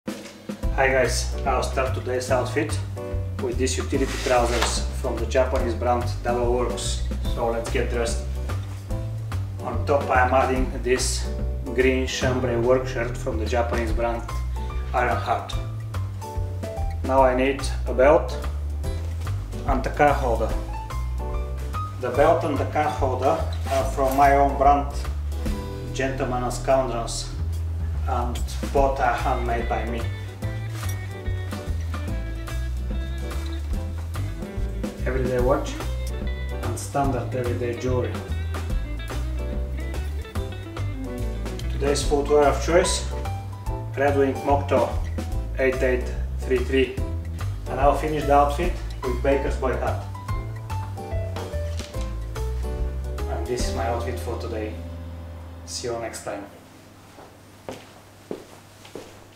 Styling Red Wing Moc-Toe 8833, Utility Trousers And Chambray Shirt | Rugged Style | GRWM
Night out rugged casual outfit